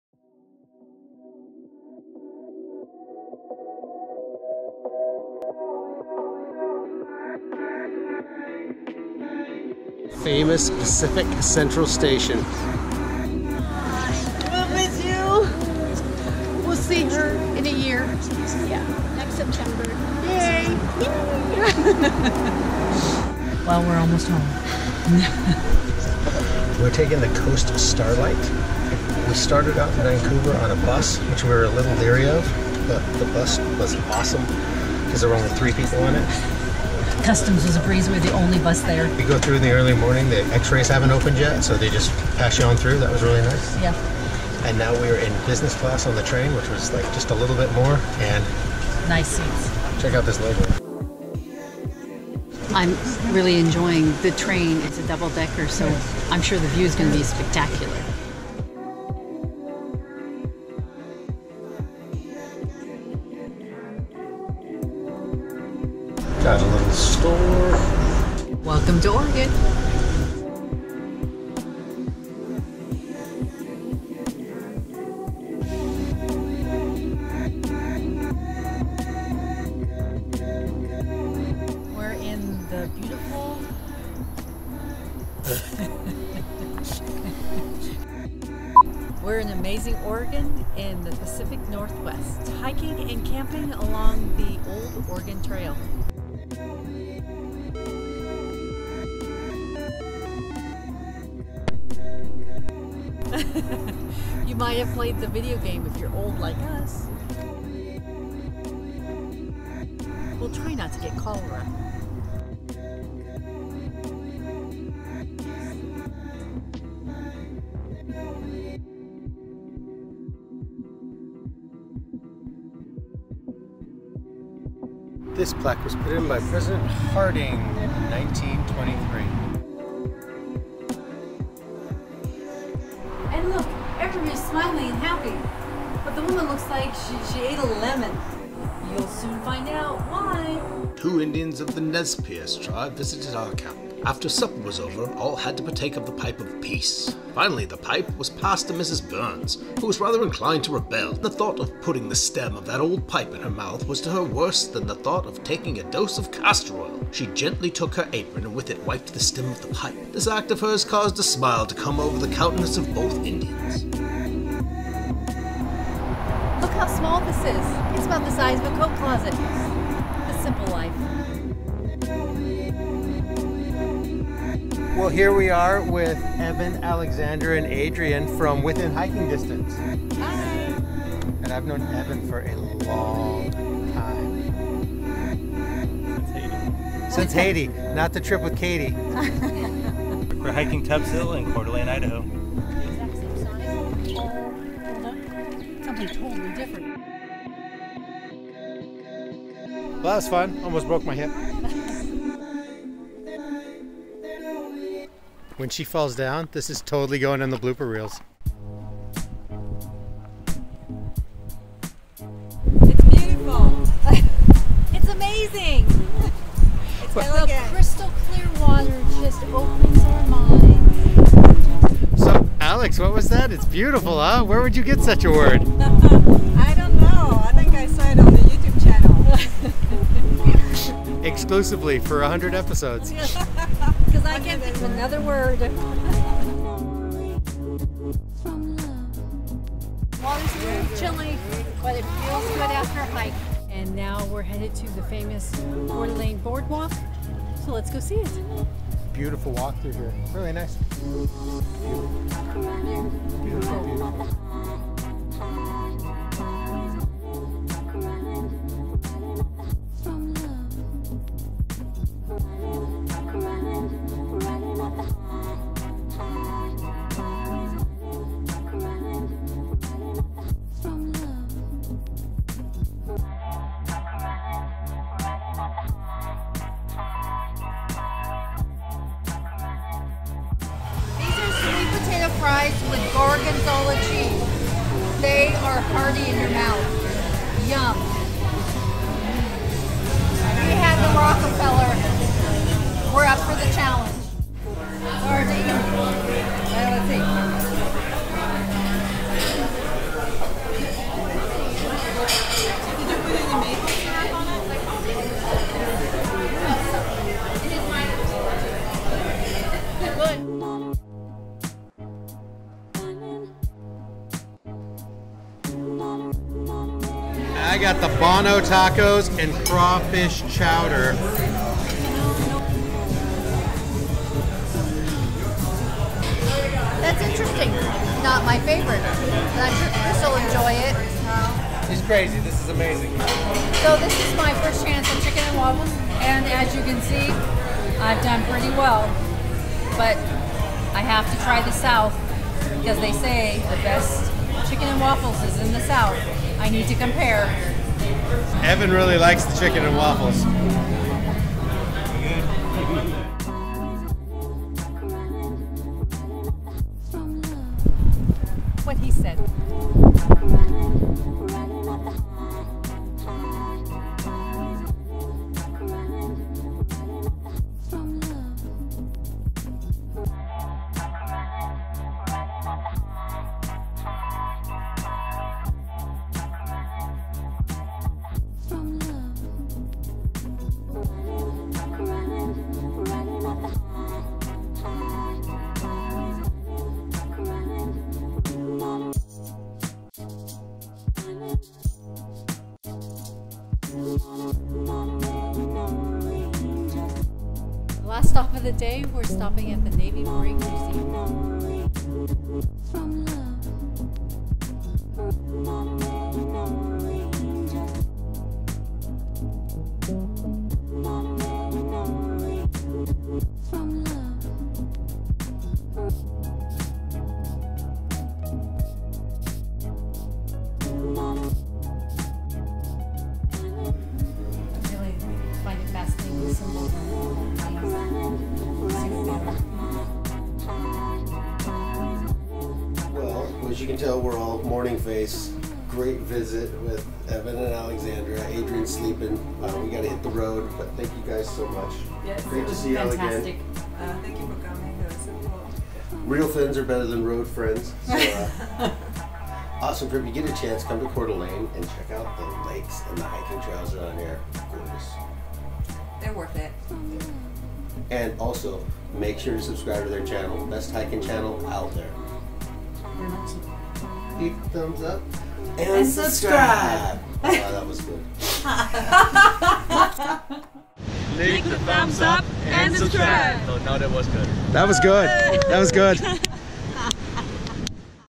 USA \\ AMTRAK Coast Starlight & HIKING w/ our favorite YouTubers // #136
We bid farewell to our amazing friends in gorgeous British Columbia and board the Amtrak Coast Starlight line to Oregon where we enjoy a day of scenic views and spectacular nature.  We drive to Idaho and along the route hike and camp along parts of the historic Oregon Trail.  We even throw in a little history 😃 We finish up in Coeur d'Alene Idaho where we meet up with some of our amazing friends who run the best hiking channel on YouTube, "Within Hiking Distance" here's the link to their awesome channel:
After hiking all over this BEAUTIFUL area, we hit the town for some of the best Taphouse cuisine around!